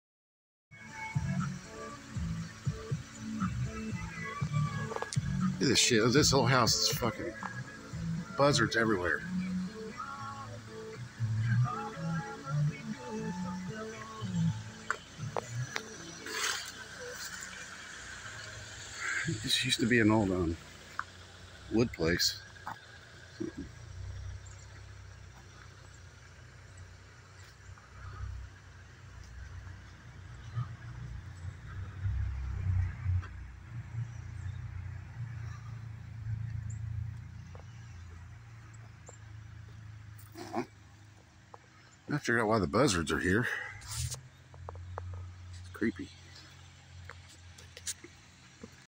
this was pretty creepy never seen anything like it
flock of buzzards on a fallen structure